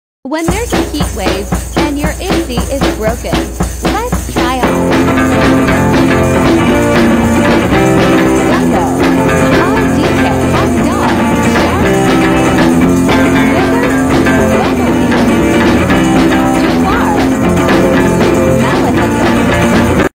Which costume is your favorite?
I took Murphy to go try on Halloween costumes and use the pet store's air conditioning during a heat wave.